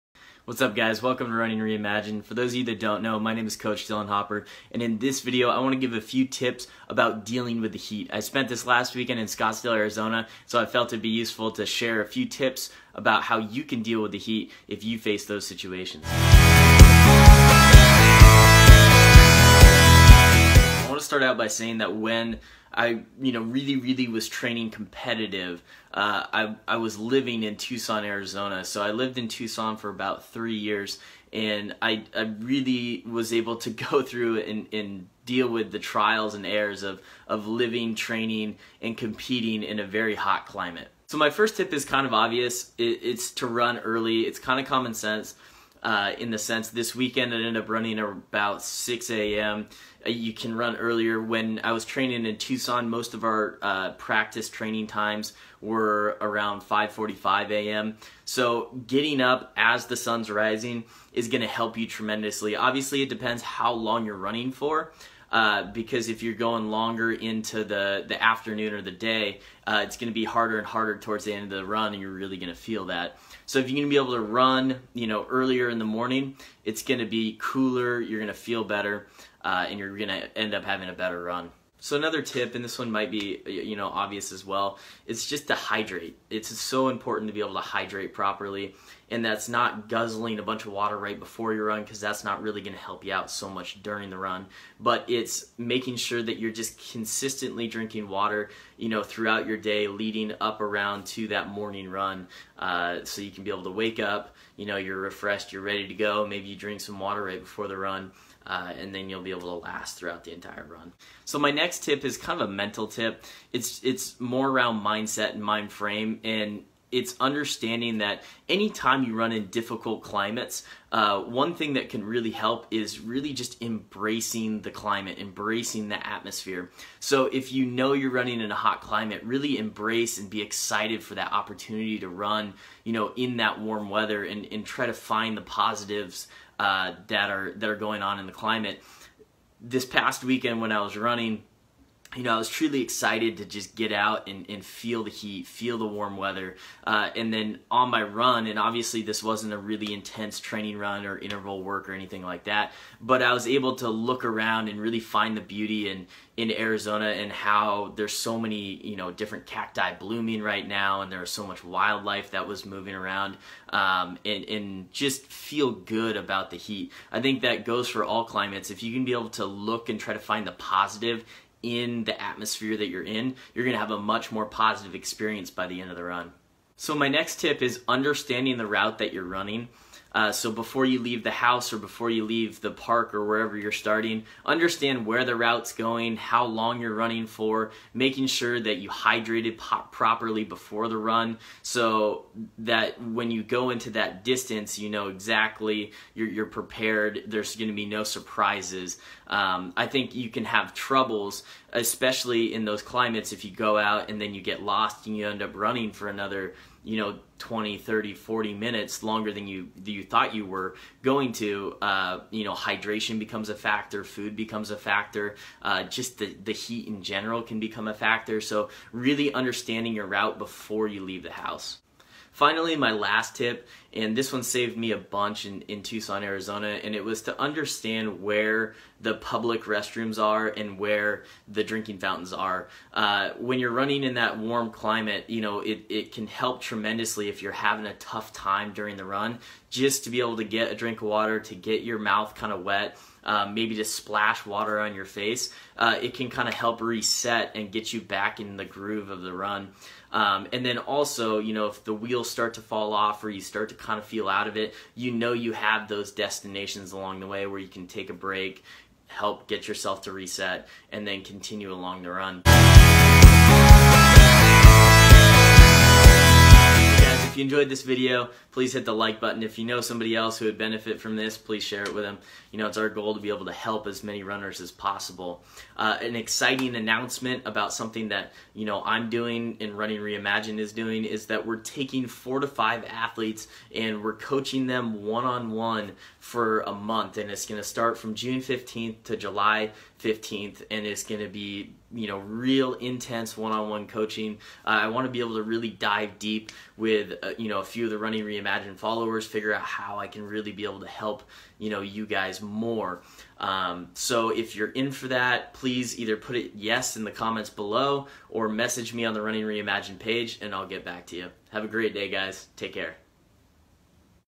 Tips for Running in HOT Climates!!!
Running in extreme climates can be difficult but if you prepare for them properly they don't have to be. In this video, several tips are discussed for Running in HOT climates.